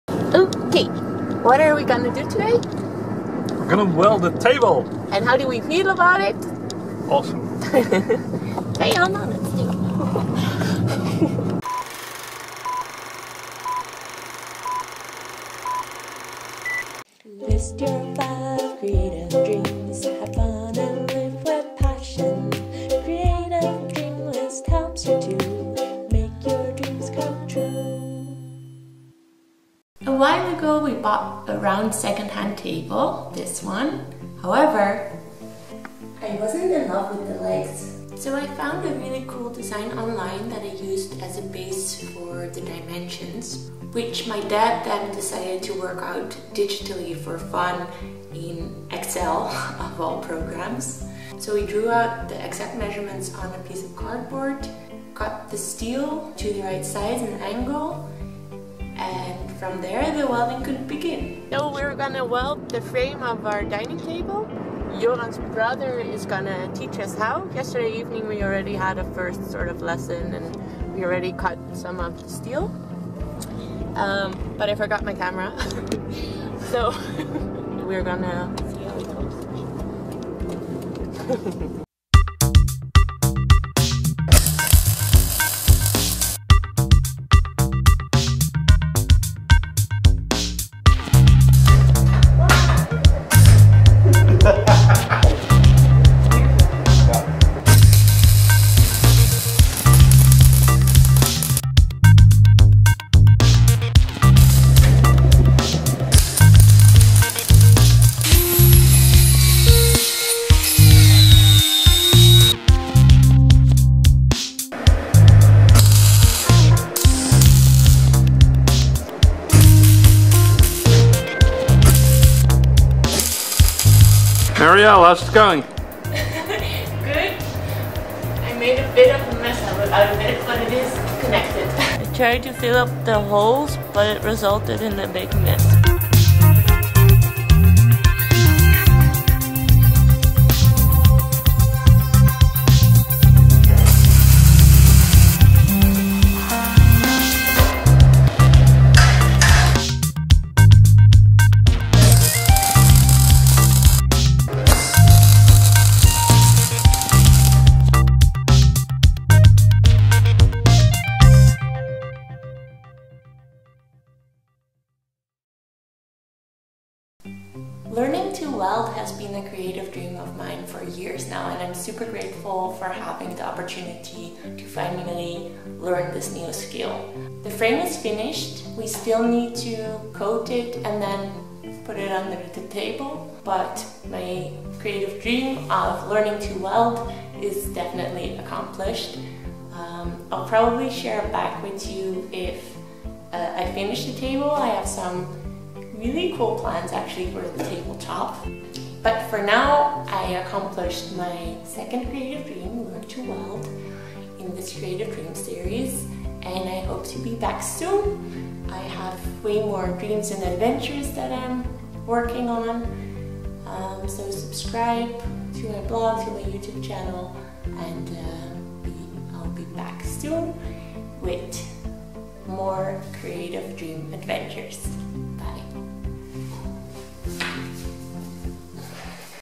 Learn to weld
I learned to weld. My husband's brother taught us how. We created a metal frame for our dining room table.